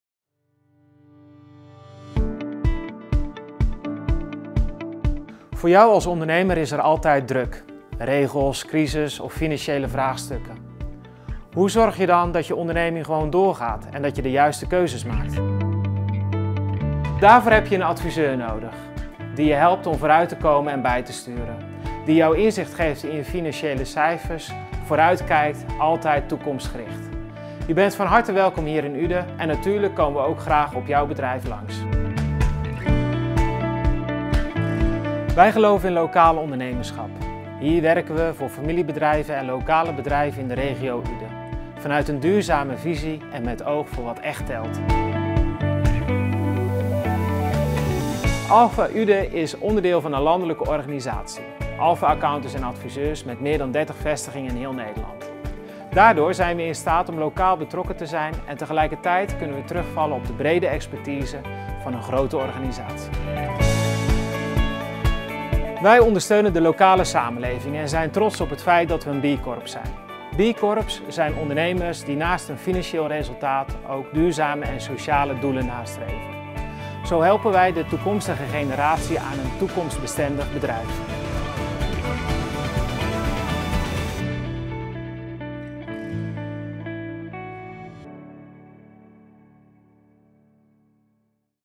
Alfa Uden - De kracht van lokaal ondernemerschap
Alfa Accountants en Adviseurs helpt al 80 jaar ondernemers bij hun bedrijfsvoering. Met meer dan 1.200 medewerkers en meer dan 35 vestigingen zijn wij overal dichtbij.

Wij van Alfa geloven in de kracht van het lokale ondernemerschap. De motor van de Nederlandse economie wordt daar draaiend gehouden. Door een samenspel van gedreven ondernemers en het netwerk van betrokken families, enthousiaste medewerkers en veel toeleverende bedrijven.